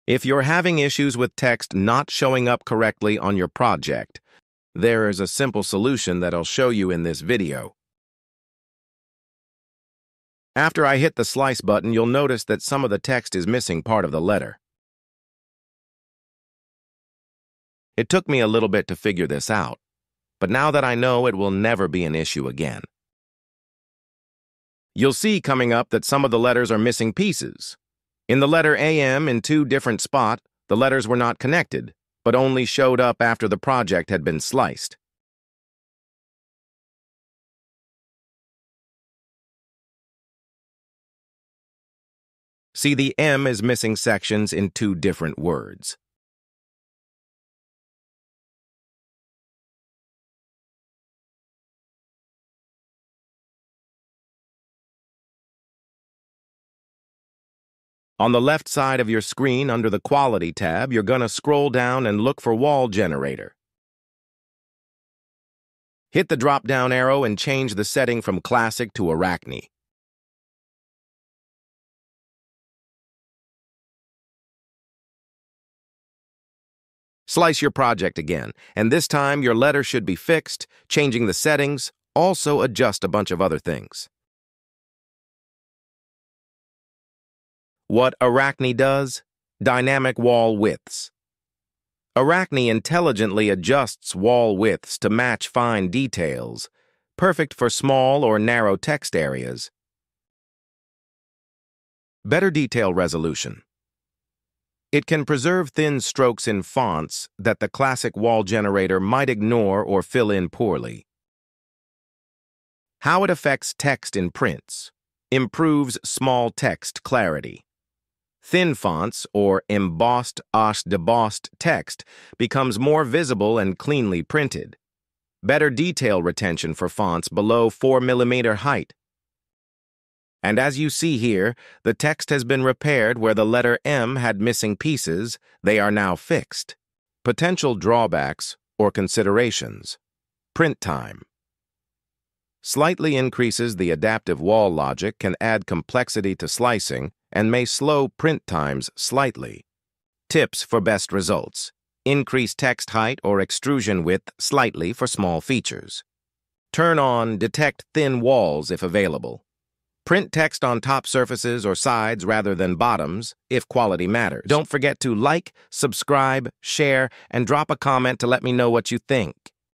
Classic vs Arachne in Bambu Studio – Fixing Text on My Michael Myers Stand!
Are you getting weird or broken text in your 3D prints? In this video, I show you how switching from Classic to Arachne wall generator settings in Bambu Studio can make a huge difference—especially for embossed or debossed text!

I used my Halloween 6: The Curse of Michael Myers display stand printed on the Bambu A1 as an example. Watch how the Arachne setting helped clean up the lettering and brought the design to life. It’s a simple change that can save your print!

🧠 Pro Tip: Arachne adapts wall widths dynamically to better preserve small details—perfect for fine text and logos.

🎃 Model Shown: Halloween 6 Michael Myers Stand
🖨️ Printer: Bambu Lab A1
🔧 Slicer: Bambu Studio
🧱 Setting Changed: Wall Generator – Classic ➡ Arachne